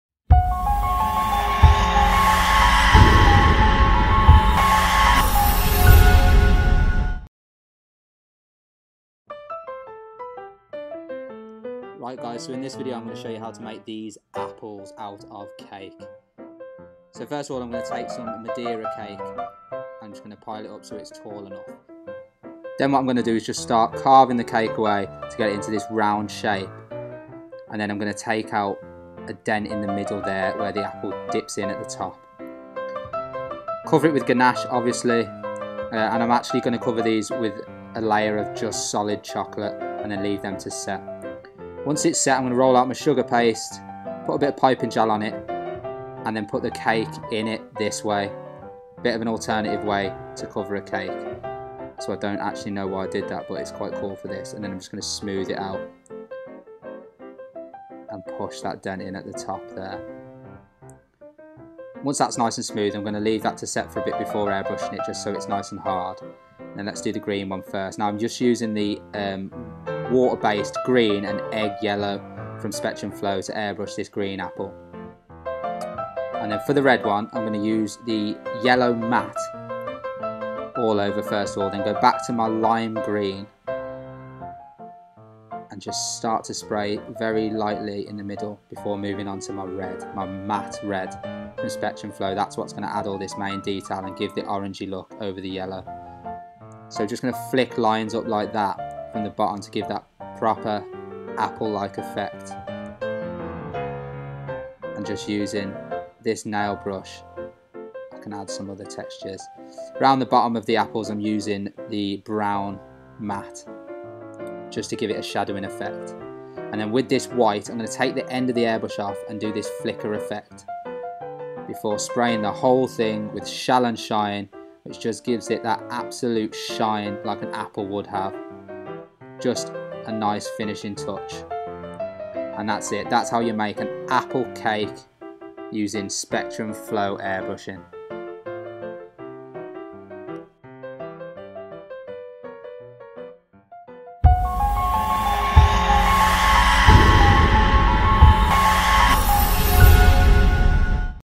How to make and airbrush a realistic apple cake!
The BakeKing uses Spectrum Flow to make this incredible edible apple fruit cake!

FREE UK delivery on all orders over £40. International shipping also available.